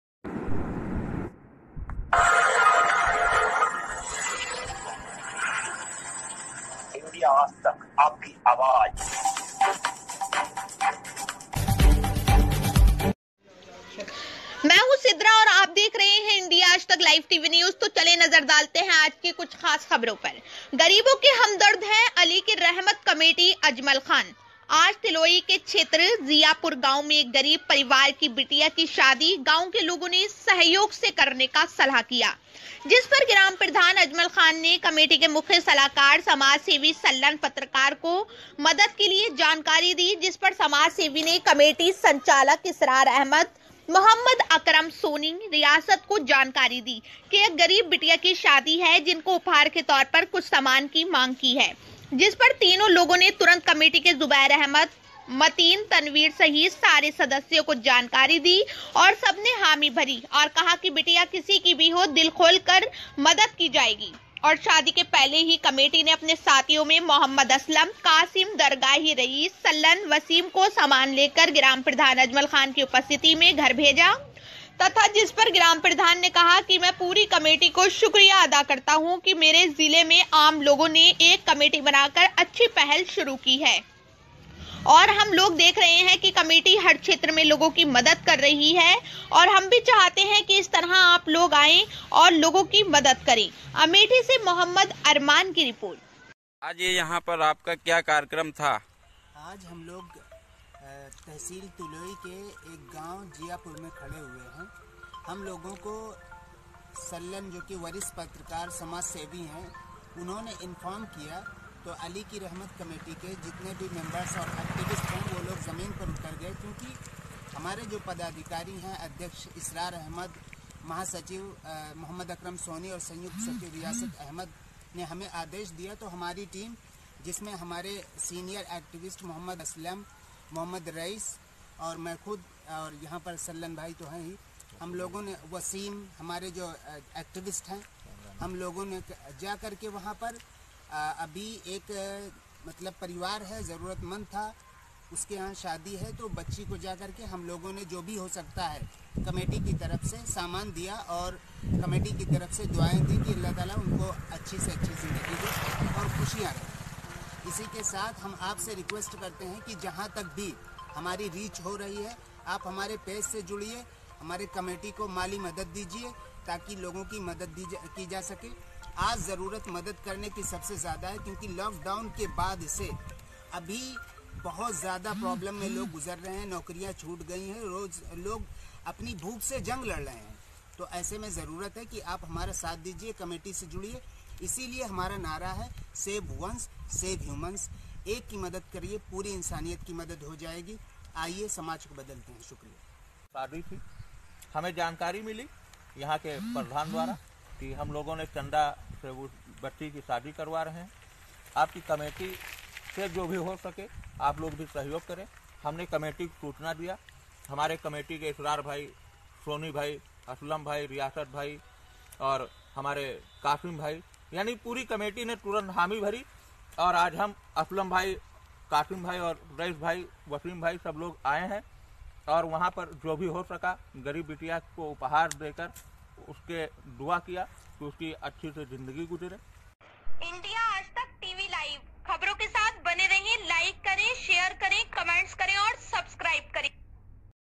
Live . गरीबों के हमदर्द है अली की रहमत कमेटी- अजमल खान आज
गरीबों के हमदर्द है अली की रहमत कमेटी- अजमल खान

आज

तिलोई के क्षेत्र जियापुर गांव में एक गरीब परिवार की बिटिया की शादी गांव के लोगों ने सहयोग से करने का सलाह किया। जिस पर ग्राम प्रधान अजमल खान ने कमेटी के मुख्य सलाहकार समाजसेवी सल्लन पत्रकार को मदद के लिए जानकारी दी। जिस पर समाजसेवी ने कमेटी संचालक इसरार अहमद, मोहम्मदअकरम  सोनी, रियासत को जानकारी दी कि एक गरीब बिटिया की शादी है जिनको उपहार के तौर पर कुछ सामान की मांग की है। जिस पर तीनों लोगों ने तुरंत कमेटी के जुबैर अहमद,मोतीन, तनवीर सहित सारे सदस्यों को जानकारी दी और सब ने हामी भरी और कहा कि बिटिया किसी की भी हो दिल खोलकर मदद की जायेगी। और शादी के पहले ही कमेटी ने अपने साथियों में मोहम्मदअसलम, कासिम दरगाही,रईश,सल्लन,वसीम को सामान लेकर ग्राम प्रधान अजमल खान की उपस्थिति में घर भेजा तथा जिस पर ग्राम प्रधान ने कहा कि मैं पूरी कमेटी को शुक्रिया अदा करता हूं कि मेरे जिले में आम लोगों ने एक कमेटी बनाकर अच्छी पहल शुरू की है। और हम  लोग देख रहे हैं कि कमेटी हर क्षेत्र में लोगों की मदद कर रही हैं। और हम भी चाहते हैं कि इस तरह और लोग आगे आये और लोगों की मदद करें।

अमेठी से मोहम्मद अरमान की रिपोर्ट